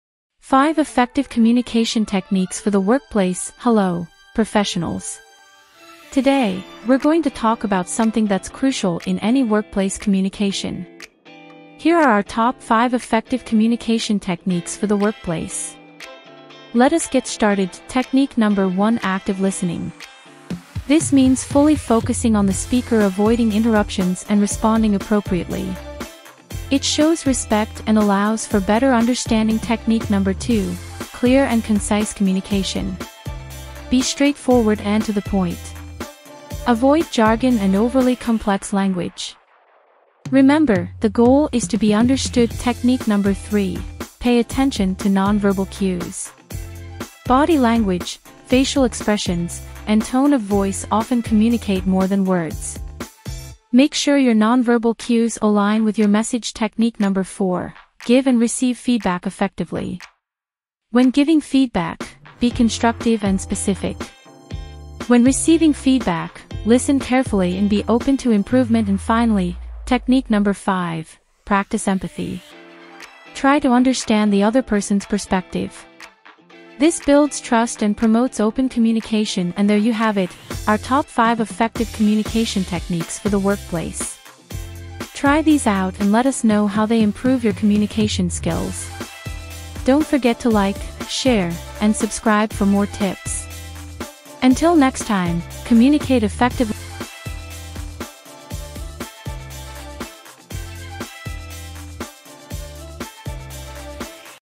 5 Effective Communication Techniques for the Workplace
"Hello, professionals! Today, we're going to talk about something that's crucial in any workplace - communication. Here are our top 5 effective communication techniques for the workplace. Let's get started!"